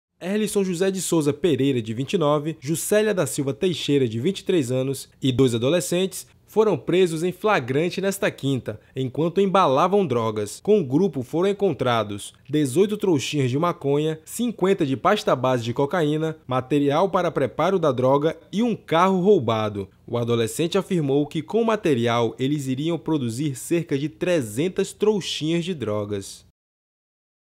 Grupo é preso em flagrante embalando drogas
Canal de vídeos do Grupo EM TEMPO de Comunicação